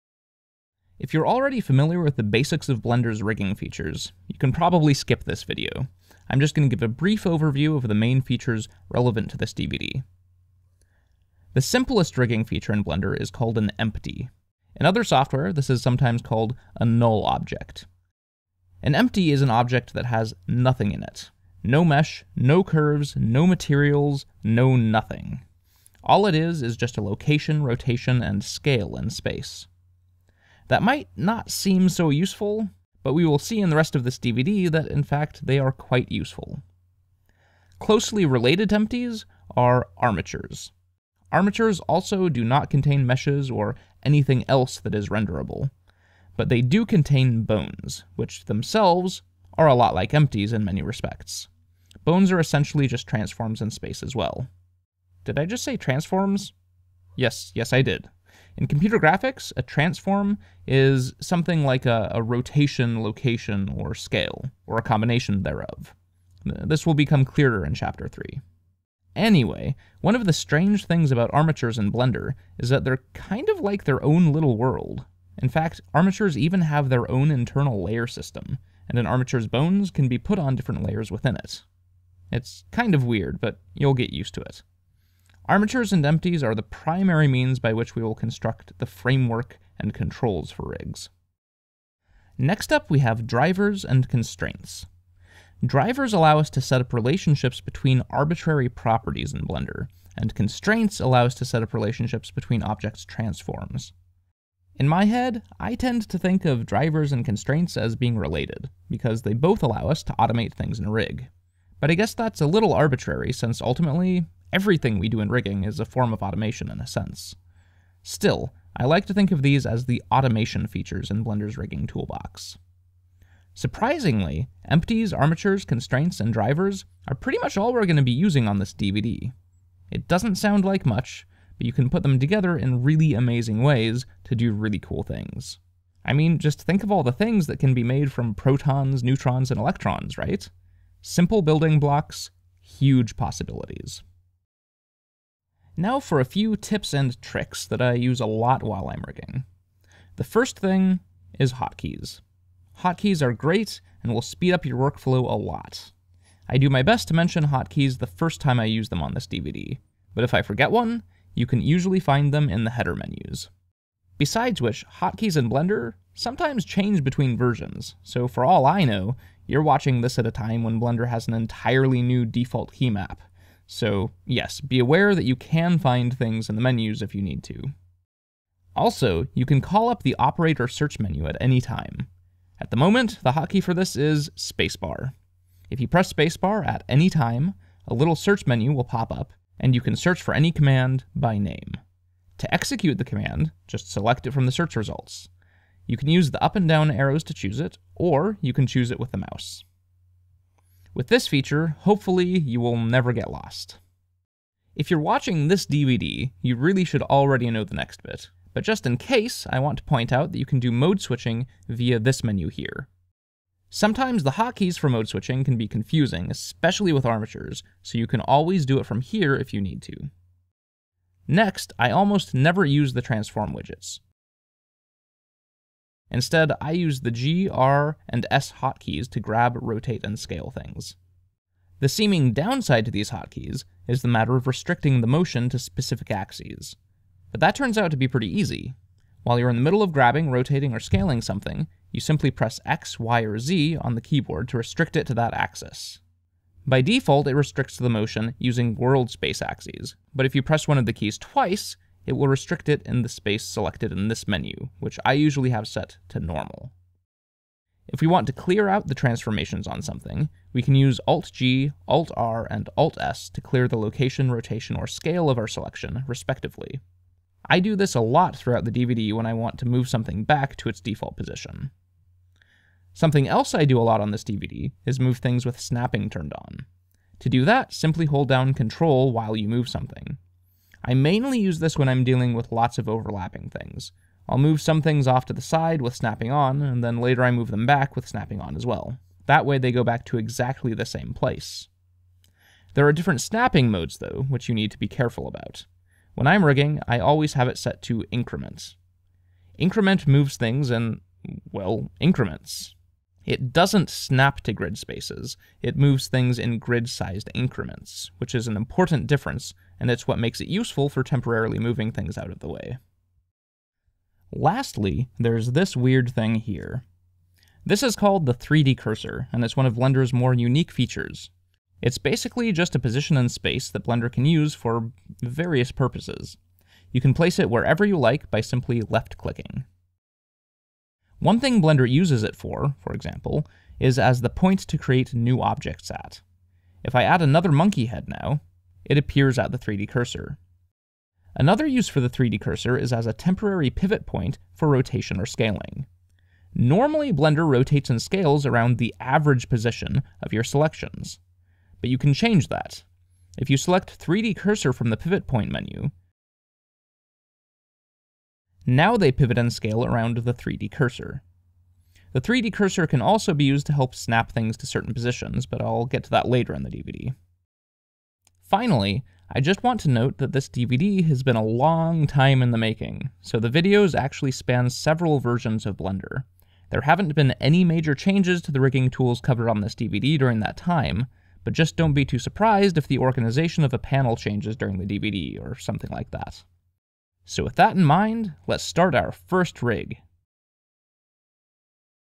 Humane Rigging: Introduction, video 02
A quick overview of some of Blender's rigging features, and a few tips on using Blender while rigging.